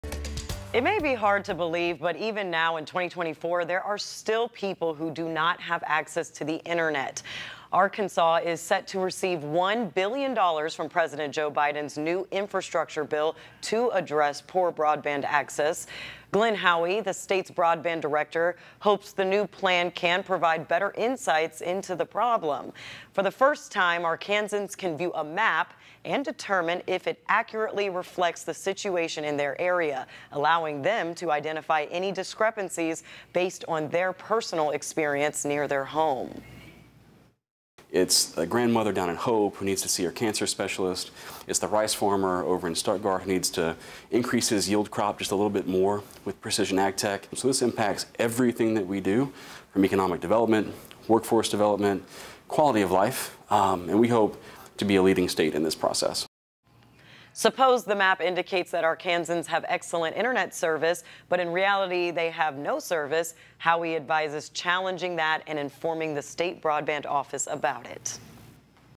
New bill could solve broadband crisis in Arkansas
Arkansas is set to receive one billion dollars from President Joe Biden's new infrastructure bill to address poor broadband access.